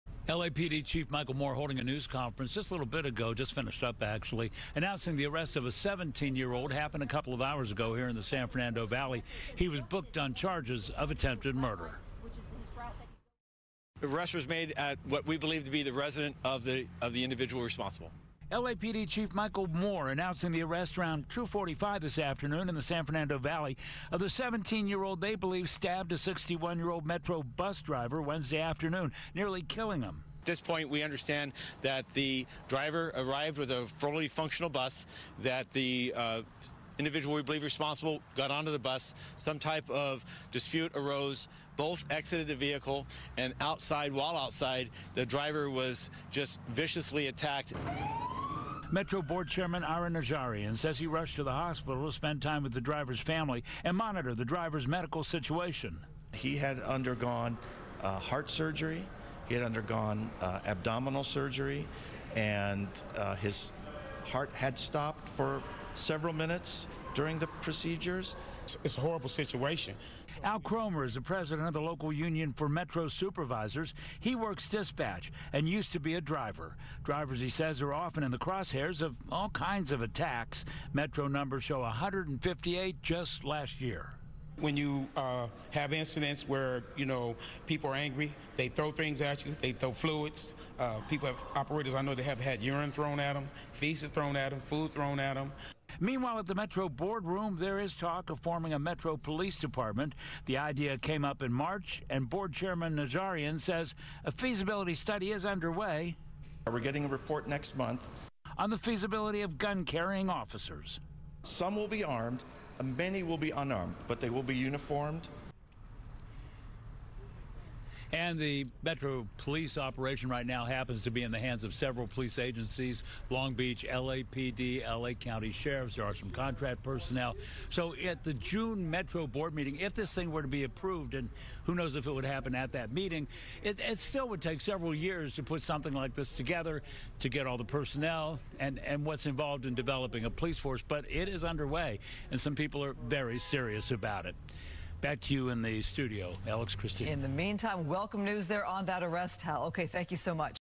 Bus driver gets heart surgery after stabbing
A Los Angeles Metro bus driver is in critical condition after getting stabbed by a teen passenger in Woodland Hills.